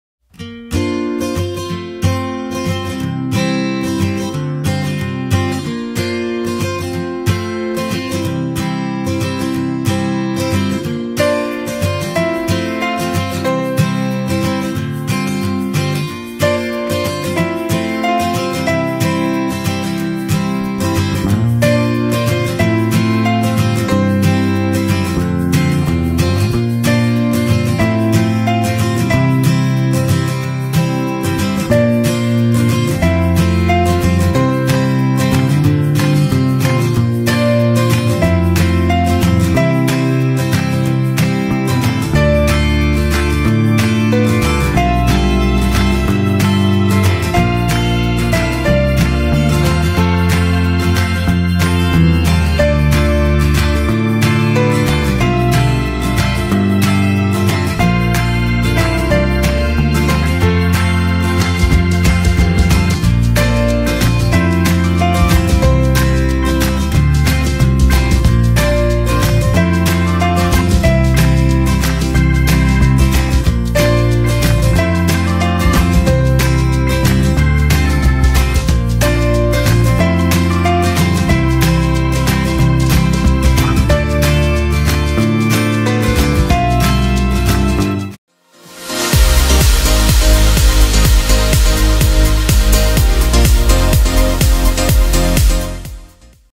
EXPLORING THE MERTHYR MAWR THATCHED ROOF COTTAGES AND SAND DUNES
Take a look at this beautiful little village near Bridgend Wales. The cottages are stunning and the sand dunes were used in the film Lawrence of Arabia. Hope you enjoy.